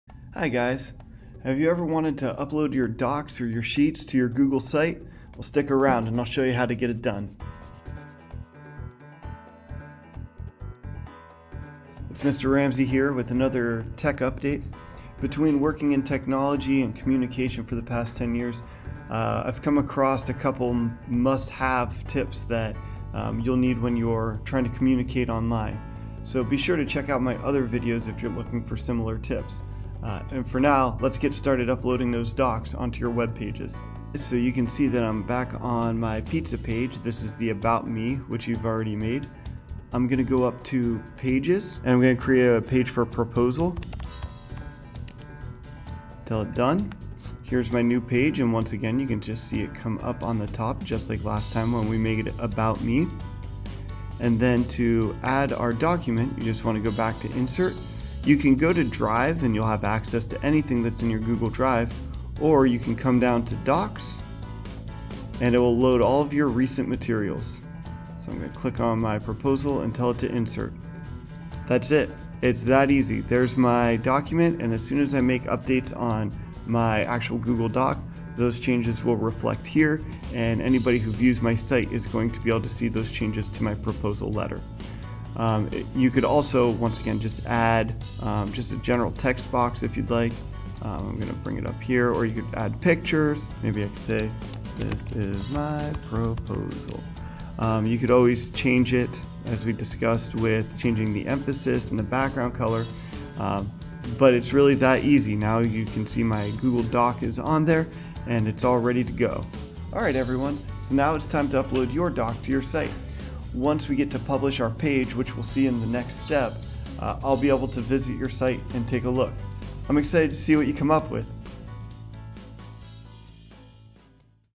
Upload a Doc to Google Sites
This short video tells you how to upload a google doc to your google site